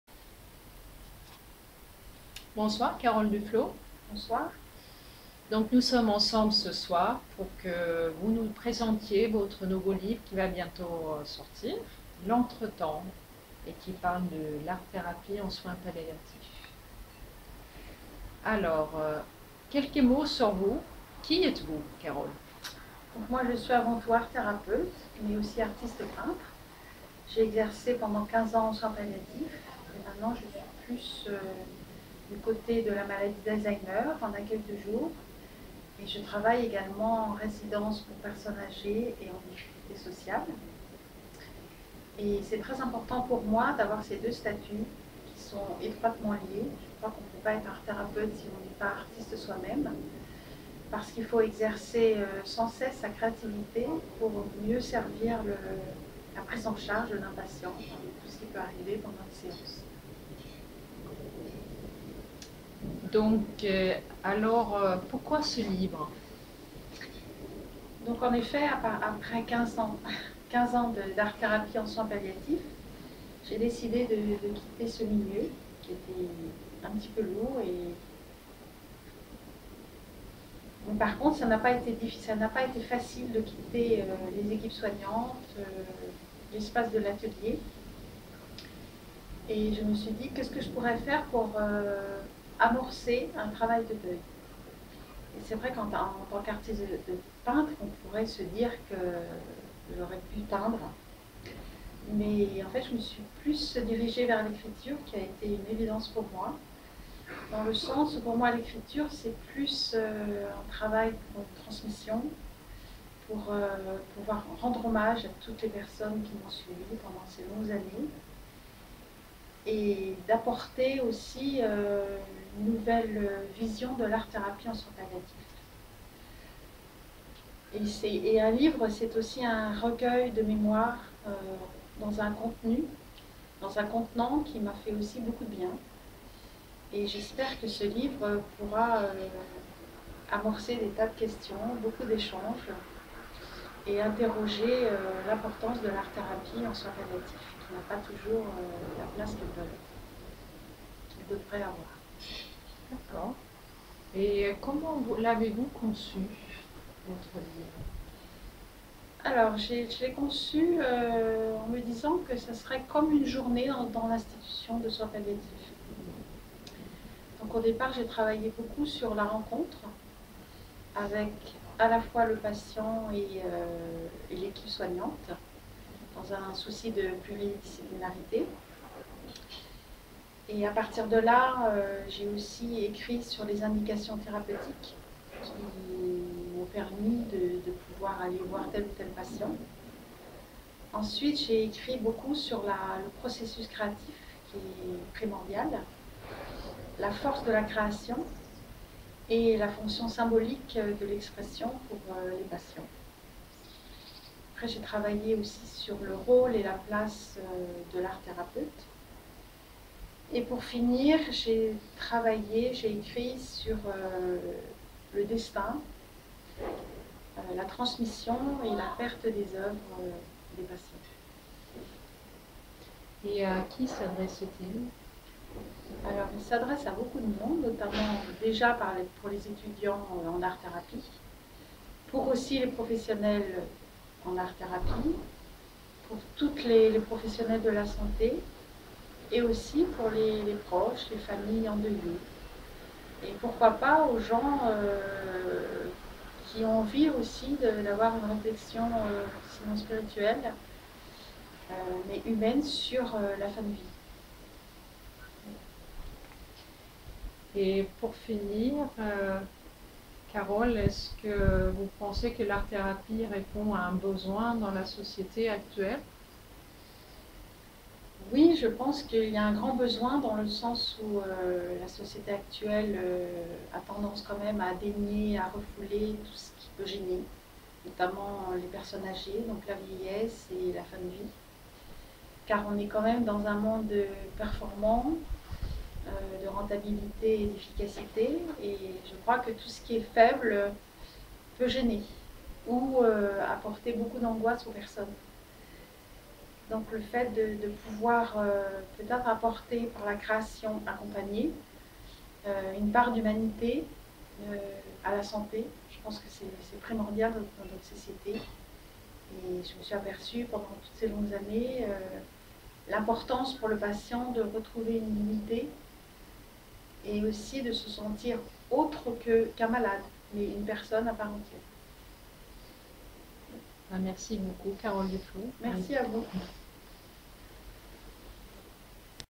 DUFLOT Carol : L'art-thérapie en soins palliatifs
« La fin de vie, c’est d’abord la vie. En unité de soins palliatifs, il ne s’agit pas d’attendre la mort, mais d’être toujours dans la vie, malgré la souffrance, malgré la colère et le chagrin. Encore quelques jours ou quelques semaines de vie, mais il peut y avoir tellement de choses à vivre et à partager avec les autres. Choisir une feuille, un crayon, un pinceau, trouver une couleur, puis une autre et donner du sens à ce que l’on représente. Travailler un bloc de terre pour le transformer en la forme unique, symbole de ce que l’on vit. En exprimant par la création l’intensité de ce qu’il ressent, le malade s’ouvre au monde qui l’entoure, à ses proches mais aussi aux soignants, aux médecins. Il peut crier sa colère, livrer son angoisse, évoquer ses symptômes. Il peut exprimer simplement ce qu’il ne parvient pas à dire.

Le travail de l’art thérapeute est d’accompagner le malade dans la création mais ce n’est pas que cela. Dans un univers où la pluridisciplinarité est essentielle, toutes les informations qui nous sont données par l’expression artistique sont essentielles à la prise en charge globale de la souffrance du malade. En côtoyant Carol Duflot, j’ai appris à comprendre cela. » E. de L.